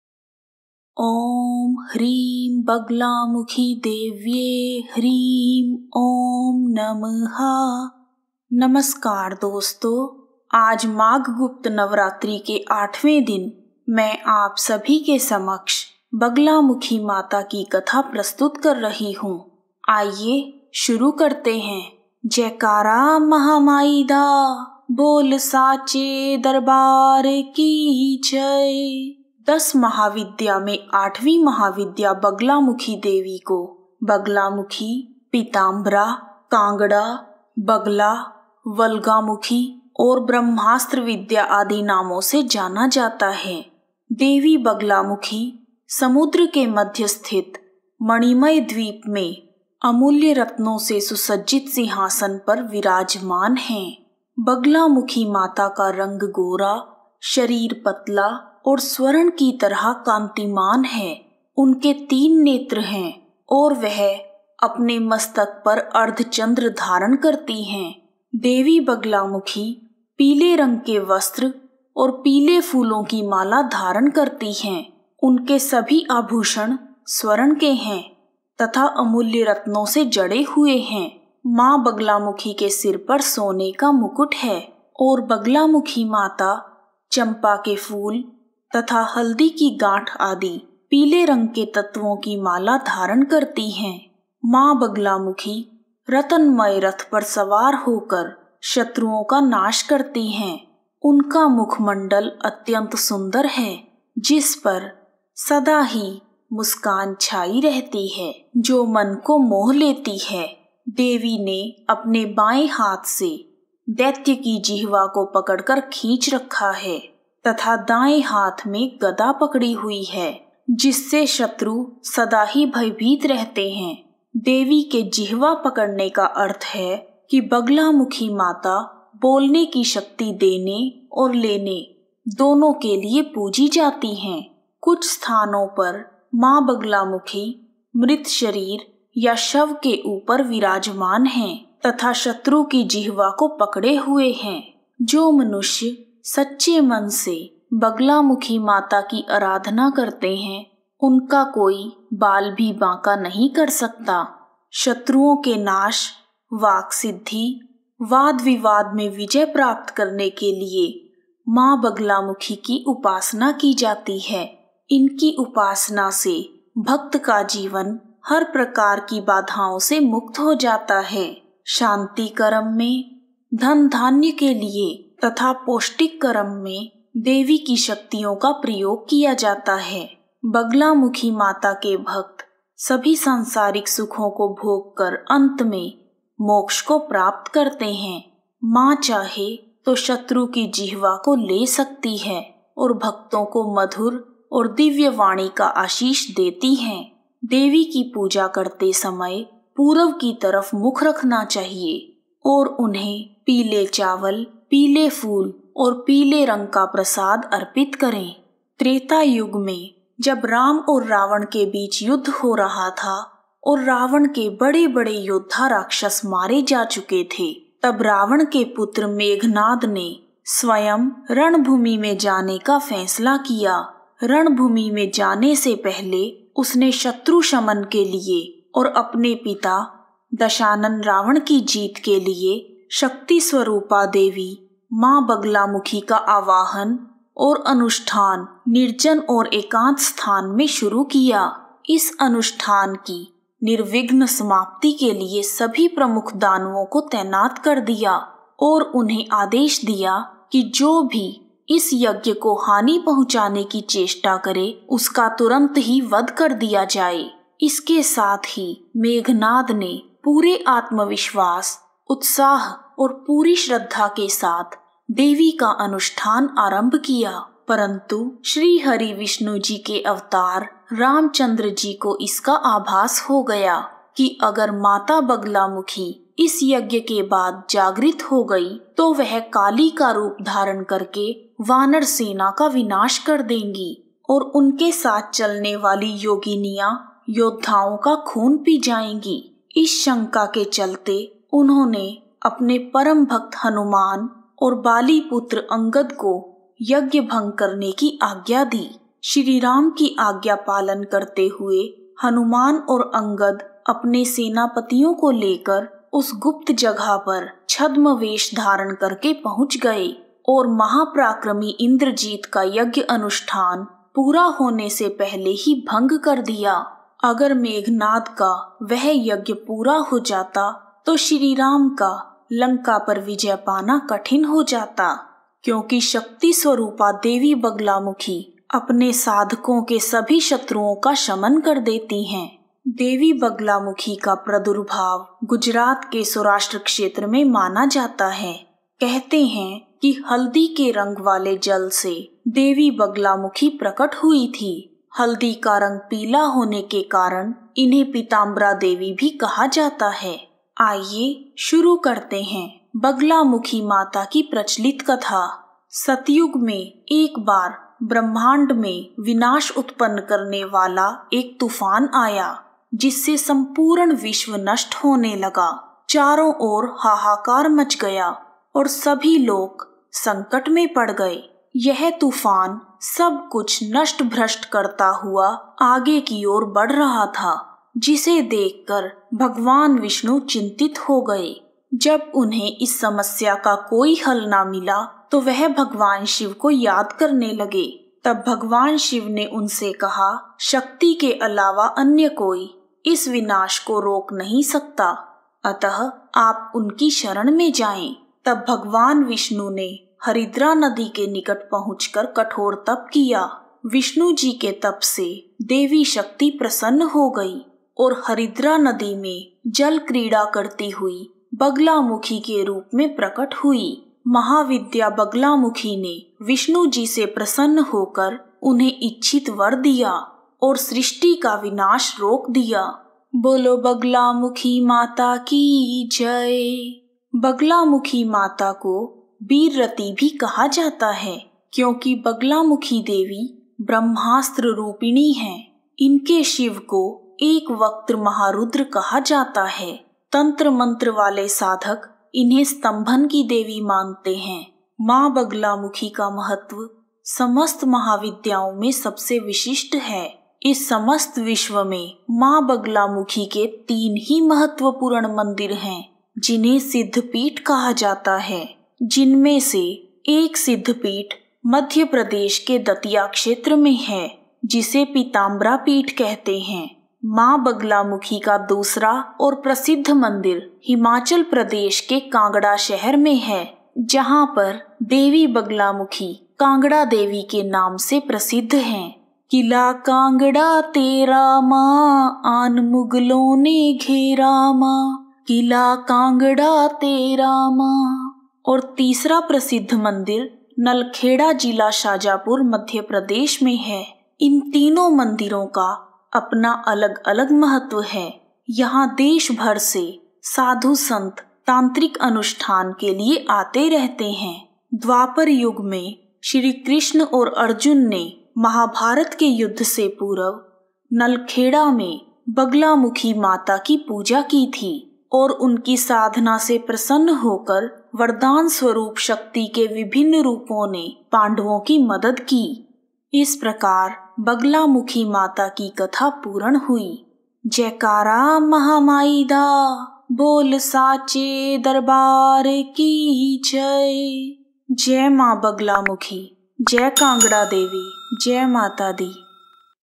माघ गुप्त नवरात्रि के आठवें दिन महाविद्या बगलामुखी माता की कथा | Gupt Navratri Bagalamukhi ki katha
ऊँ ह्नीं बगुलामुखी देव्यै ह्नीं ओम नम:
नमस्कार दोस्तों आज 19 फरवरी 2021माघ गुप्त नवरात्रि के आठवें दिन मैं आप सभी के समक्ष 10 महाविद्या में आठवीं महाविद्या बगलामुखी माता गुप्त नवरात्रि की कथा प्रस्तुत कर रही।आइए शुरू करते हैं बगलामुखी माता की कथा  आठवीं महाविद्या बगलामुखी देवी की कथा  मां बगलामुखी की कथा

 दस महाविद्याओं में आठवीं महाविद्या बगलामुखी देवी को बगलामुखी, पीताम्बरा, बगला, वल्गामुखी, वगलामुखी, ब्रह्मास्त्र विद्या आदि नामों से भी जाना जाता है।देवी बगलामुखी, समुद्र के मध्य में स्थित मणिमय द्वीप में अमूल्य रत्नों से सुसज्जित सिंहासन पर विराजमान हैं। बगलामुखी माता का रंग गोरा,  शरीर पतला और स्वर्ण की तरह कांतिमय है । उनके  तीन नेत्र हैं और वह अपने मस्तक पर अर्ध चन्द्र धारण करती है। देवी  बगलामुखी ने पीला वस्त्र तथा पीले फूलों की माला धारण की हुई है। देवी के सभी आभूषण स्वर्ण के हैं तथा अमूल्य रत्नों से  जड़े हुए हैं। मां बगलामुखी के सिर पर सोने का मुकुट है और मां चंपा के फूल तथा हल्दी की गांठ आदि पीले रंग से सम्बंधित तत्वों की माला धारण करती हैं।  बगलामुखी माता रत्नमय रथ पर  सवार होकर शत्रुओं का नाश करती हैं। उनका मुख मंडल अत्यंत सुंदर है जिस पर  सदा मुस्कान छाई रहती है जो मन को मोह  लेती है ।देवी ने अपने बाएं हाथ से दैत्य  की जिह्वा को पकड़ कर खींच रखा है तथा दाएं हाथ मैं गदा  पकड़ी हुई हैं, जिससे शत्रु भयभीत हो रहा है। देवी के जिव्हा पकड़ने का अर्थ यह है कि  बगलामुखी माता  बोलने की शक्ति देने और लेने के लिए पूजी जाती हैं। कई स्थानों में देवी ने मृत शरीर या शव को अपना आसन बना रखा है तथा शव पर ही  सवार हैं तथा दैत्य या शत्रु की जिह्वा को पकड़ रखा हैं।    जो मनुष्य सच्चे मन से मां की आराधना करता है, उसका कोई बाल भी बांका नहीं कर सकता।
शत्रुनाश, वाकसिद्धि, वाद विवाद में विजय  प्राप्त करने के लिए बगलामुखी माता की उपासना की जाती है। इनकी उपासना से शत्रुओं का नाश होता है तथा भक्त का जीवन हर प्रकार की बाधा से मुक्त हो जाता है। शांति कर्म में, धन-धान्य के लिए, पौष्टिक कर्म में, वाद-विवाद में विजय प्राप्त करने हेतु देवी उपासना व देवी की शक्तियों का प्रयोग किया जाता हैं।  बगलामुखी माता की सच्चे मन से आराधना करने से मनुष्य सभी सांसारिक सुखों को भोग  कर अंत में मोक्ष  को प्राप्त करता है। मां चाहें तो शत्रु की जिव्हा ले सकती हैं और भक्तों को मधुर और दिव्य वाणी का आशीष देती हैं। देवी की पूजा करते समय पूर्व की तरह मुंह करें। बगलामुखी देवी को समर्पित करने के लिए पीले चावल, पीले फूल और पीले प्रसाद का प्रयोग करें।
Hello friends today, 19 February 2021, on the eighth day of Magha Gupta Navratri, I am presenting the story of Eighth Mahavidya Bagalamukhi Mata Gupt Navratri to all of you in 10 Mahavidya. Let's begin Bagalamukhi Mata ki Katha  Bagalamukhi mata ki kahani

 Baglamukhi Devi, the eighth of the ten Mahavidyas, is also known by the names Bagalamukhi, Pitambara, Bagla, Vulgamukhi, Vagalamukhi, Brahmastra Vidya, etc. Devi Bagalamukhi sits on a throne decorated with priceless gems in the beautiful island in the middle of the sea. Bagalamukhi Mata has fair complexion like blonde, slim body and golden. She has three eyes and holds a crescent moon on her forehead. Goddess Baglamukhi is wearing a yellow robe and a garland of yellow flowers. All the deities of the goddess are of gold and are studded with priceless gems. Mother Bagalamukhi has a gold crown on her head and mother holds a garland of elements related to yellow color like Champa flowers and turmeric lumps. Baglamukhi Mata destroys enemies by riding on the Ratnamayi Chariot. His face is very beautiful, which always has a smile that captivates the mind. Devi has pulled the demon's tongue with her left hand and is holding the mace in the right hand, which is frightening the enemy. . The meaning of holding the tongue of the goddess is that Bagalamukhi Mata is worshiped to give and take the power of speaking. In many places, the goddess has kept the dead body or body as her seat and is riding on the dead body and holding the demon or enemy's tongue. No one can stop a person who worships the mother with a sincere heart.
Bagalamukhi Mata is worshiped for victory in Shatruja, Vakasiddhi, debate. Enemies are destroyed by their worship and the life of the devotee is freed from all kinds of obstacles. Goddess worship and the powers of the Goddess are used to achieve victory in peace work, in wealth, in nutritional work, in debate. Worshiping Bagalamukhi Mata with true heart, man attains salvation by finally enjoying all the worldly pleasures. Mother can take the tongue of the enemy if she wishes and blesses the devotees with sweet and divine voice. When worshiping the Goddess, face as before. Use yellow rice, yellow flowers and yellow prasad to dedicate to Baglamukhi Devi.